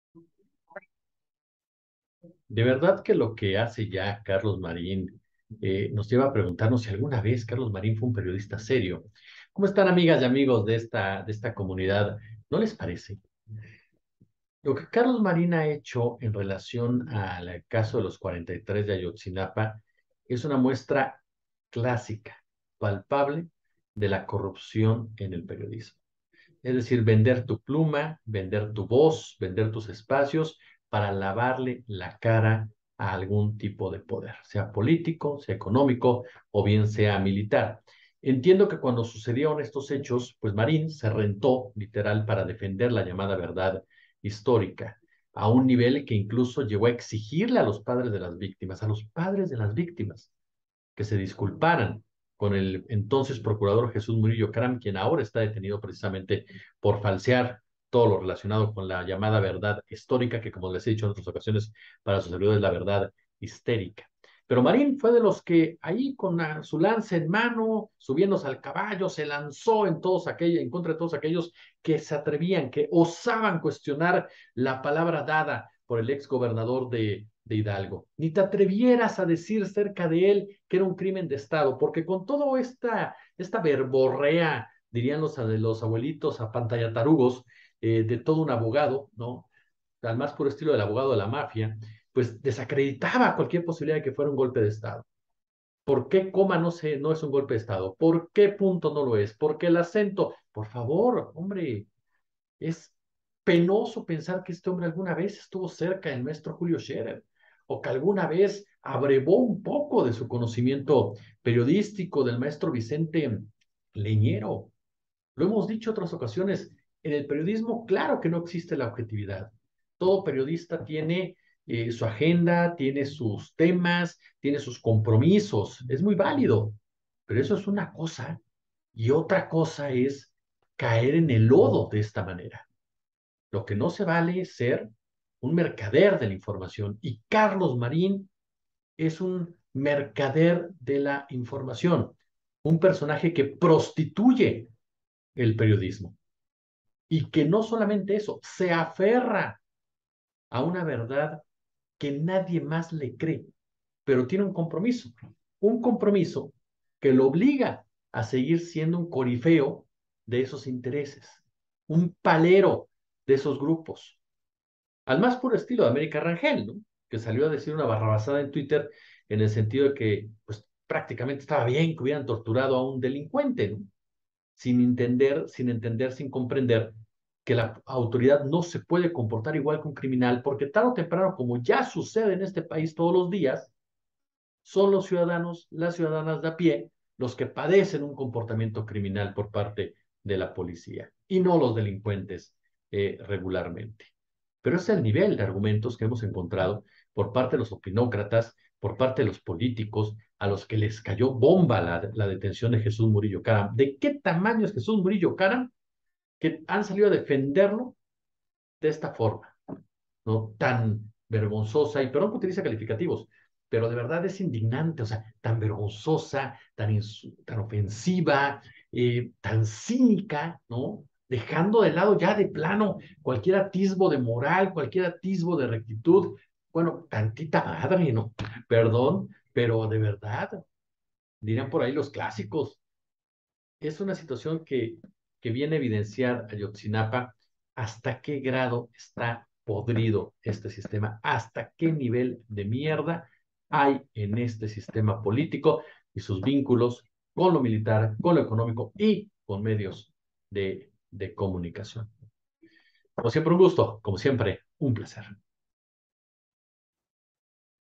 CARLOS MARÍN NO TIENE MADRE, PROSTITUYÓ la verdad de AYOTZINAPA por DINERO: Jesús Escobar
“Marín se rentó para defender la llamada “verdad histórica” para lavarle la cara al poder político económico y militar”